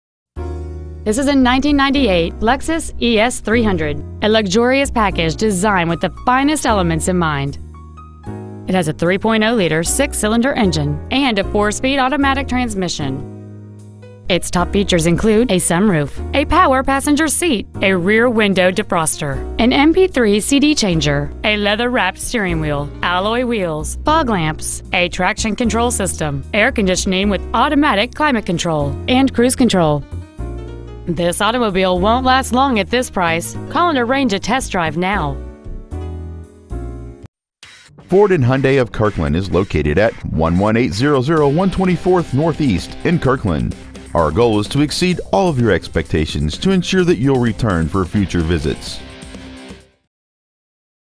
1998 Lexus ES300 Seattle WA
We are proud to present this 1998 Lexus ES300.

 Year: 1998
 Make: Lexus
 Model: ES300
 Engine: 3.0L V6
 Trans.: 4 Spd Automatic
 Exterior: Tan
 Miles: 204,793
 Interior: Gray

 Pre-Owned 1998 Lexus ES300 Kirkland WA